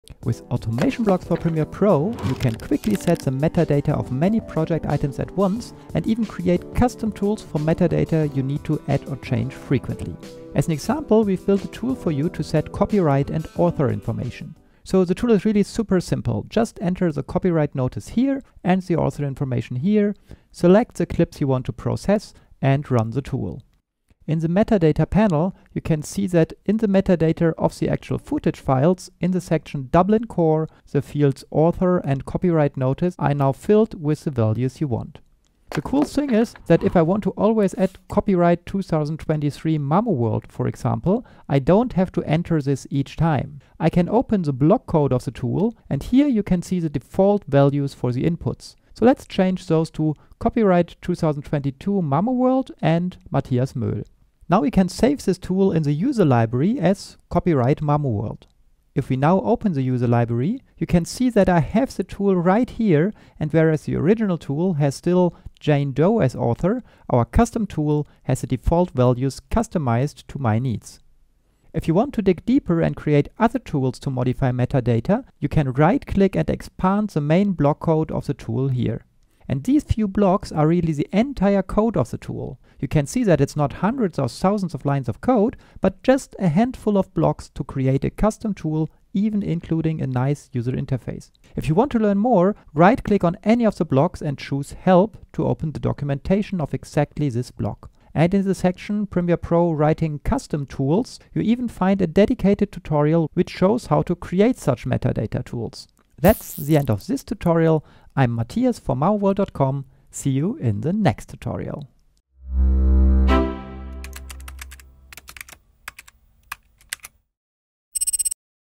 Batch Set Copyright & Author Metadata in Premiere Pro
Learn how to quickly set copyright and author metadata for multiple clips in Adobe Premiere Pro all at once! Save time and streamline your project workflow with this step-by-step tutorial.

Transcript:

With Automation Blocks for Premiere Pro you can quickly set the metadata of many project items at once and even create custom tools for metadata you need to add or change frequency.

As an example, we've built a tool for you to set copyright and author information.

In the metadata panel, you can see that in the metadata of the actual footage files in the section "Dublin Core" the fields "author" and "copyright notice" are now filled with the values we want.

Now we can save this tool in the User Library as "copyright mamoworld".
If we now open the User Library, you can see that I have the tool right here and whereas the original tool still has Jane Doe as author, our custom tool has the default values customized to my needs.

If you want to dig deeper and create other tools to modify metadata, you can right-click and expand the main block code of the tool here.
And these few blocks are really the entire code of the tool - you can see that it is not hundreds or thousands of lines of code, but just a hand full of blocks to create a custom tool even including a nice user interface

If you want to learn more, right-click on any of the blocks and choose "help" to open the documentation of exactly this block. And in the section "Pr - Writing Custom Tools" you even find a dedicated tutorial which shows how to create such metadata tools.

That's the end of this tutorial. I am Mathias for mamoworld.com - see you in the next tutorial!